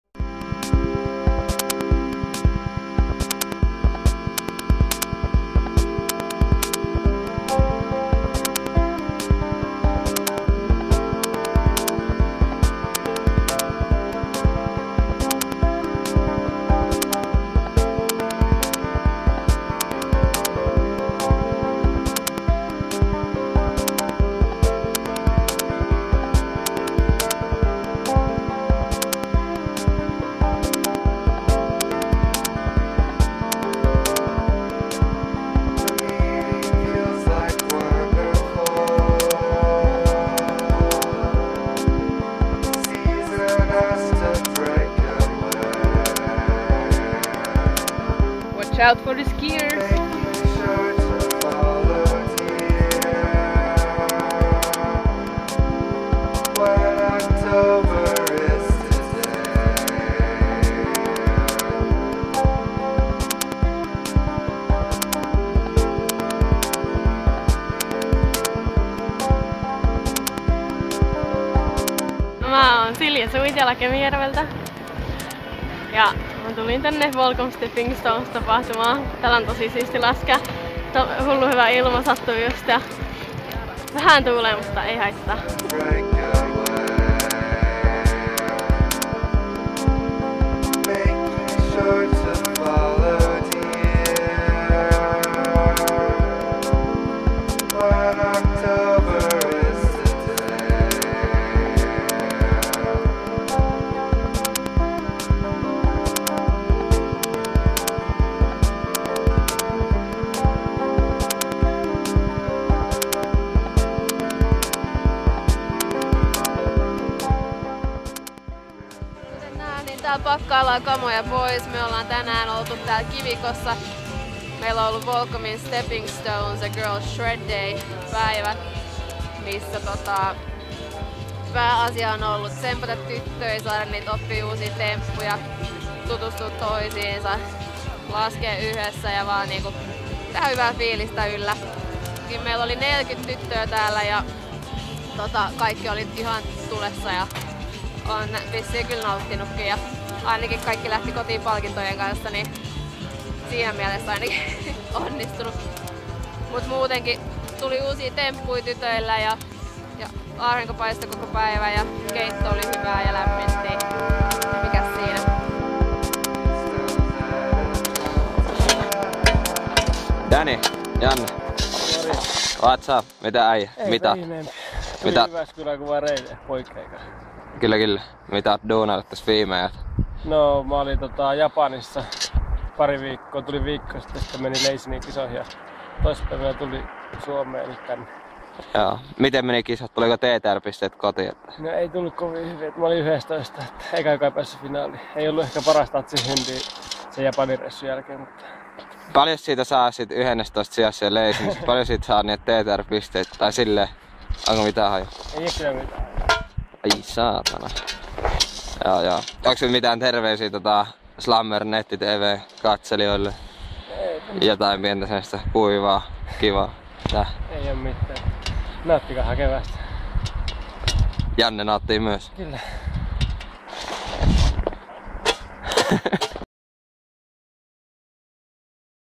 SLAMMERTV S01E02
-Volcom Stepping Stones girls shred day @ Kivikko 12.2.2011
-Janne Lipsasen kuulumiset